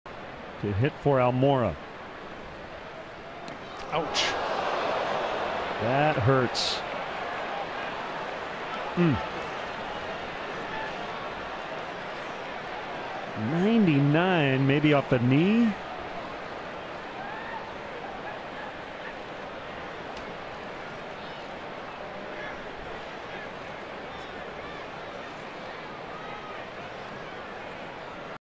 CHC@WSH: Candelario stays in game after getting hit
Jeimer Candelario stays in the game after taking a Blake Treinen pitch off his left knee